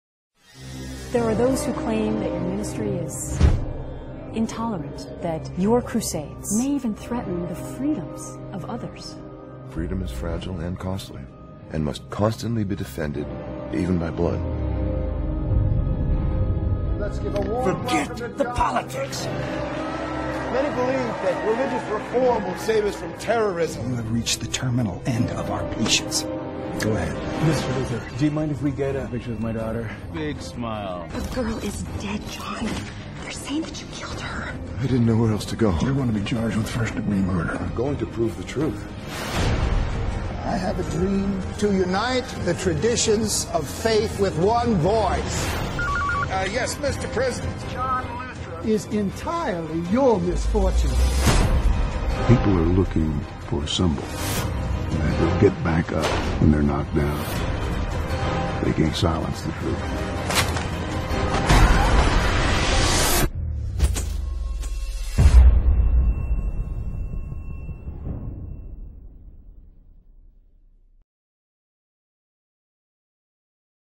'Persecuted: I Will Not Be Silent' by Robin Parrish
A storm is coming.

The most popular evangelist of his day, John Luther, simply wants to share the Gospel and enjoy a quiet life with his family. He never asked to be at the center of a political controversy, but an ill wind has been blowing through the halls of Congress, and supporters of a new religious-equality bill see Luther's endorsement as critical.

But when Luther refuses to lend his support, he unknowingly sets in motion an explosive plan bent on destroying his reputation and undermining everything for which he stands. His once-normal life is turned upside down as he becomes a fugitive left with only one dangerous option--putting everything he holds most dear at risk while he fights to expose the truth. It is a mission that brings him face-to-face with the coming storm of persecution that could threaten the very fabric of our nation's freedoms.